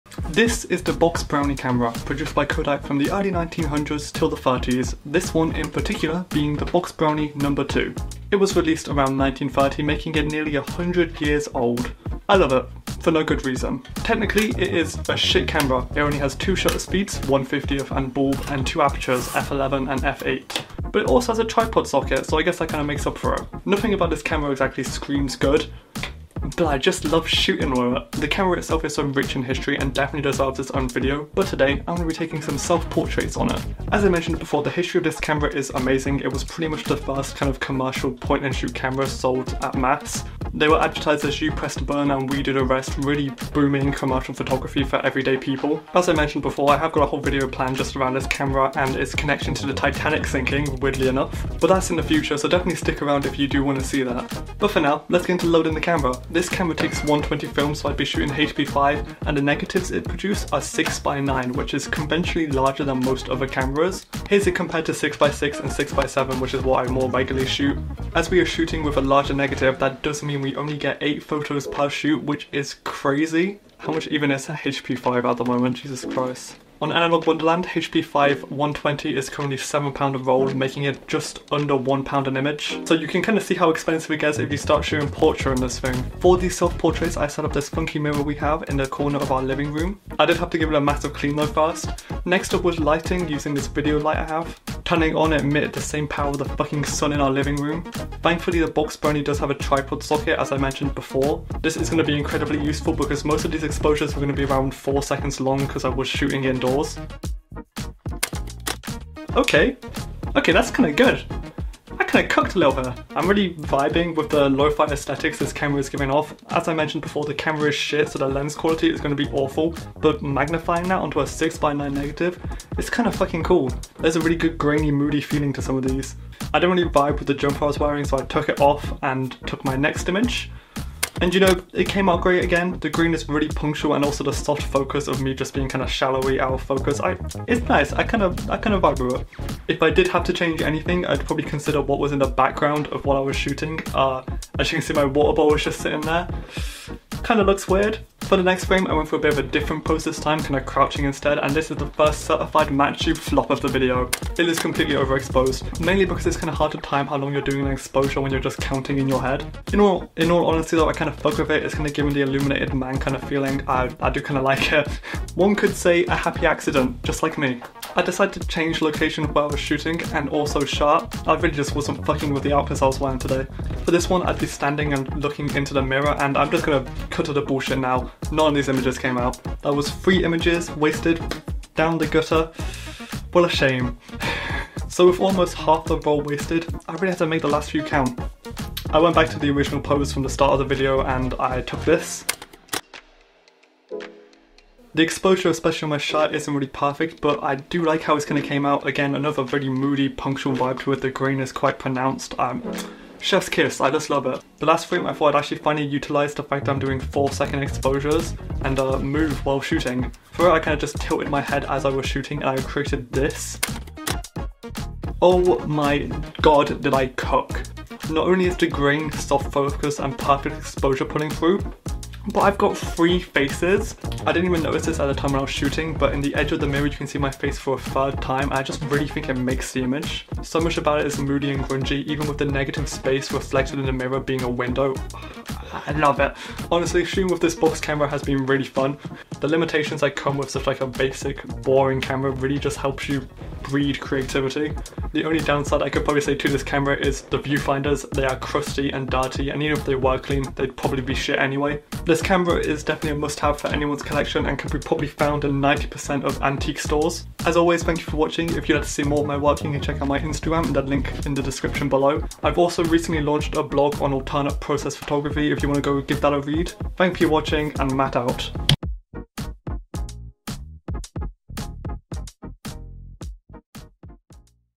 Self Portraits on a 100 year old camera
in this video i use a near 100 year old camera to take some self portraits! the camera, the kodak box brownie no2 is a medium format 6x9 camera that is a pretty limiting camera, with only 2 shutter speeds and apertures, it's amazing still being able to use this film camera so rich in history!